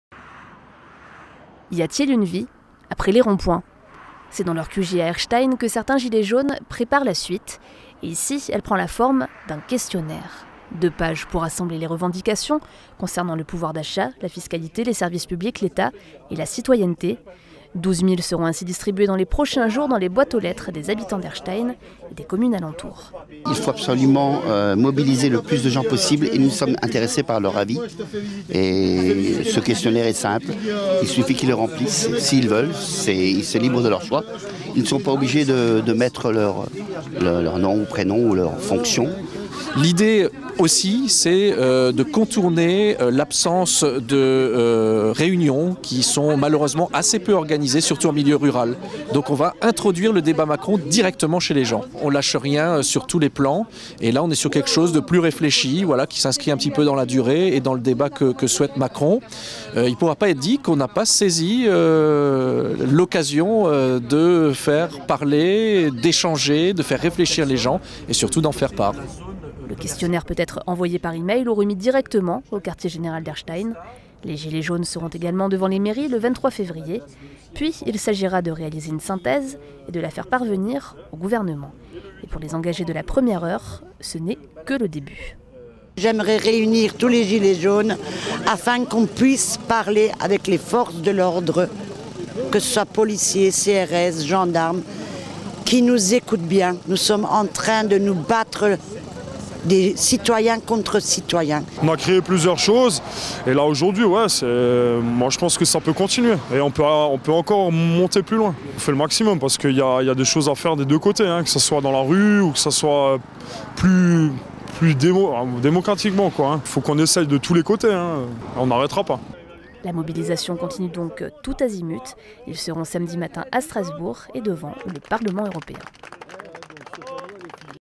Gilets jaunes, la mobilisation continue.
Après 12 semaines de manifestations. Le mouvement des gilets jaunes continue. Le prochain rendez-vous est donné samedi devant le parlement européen à Strasbourg. En marge de cette manifestation, les gilets jaunes d’Erstein et alentours déposent des questionnaires dans les boîtes aux lettres. Le but : mobiliser un maximum de citoyens.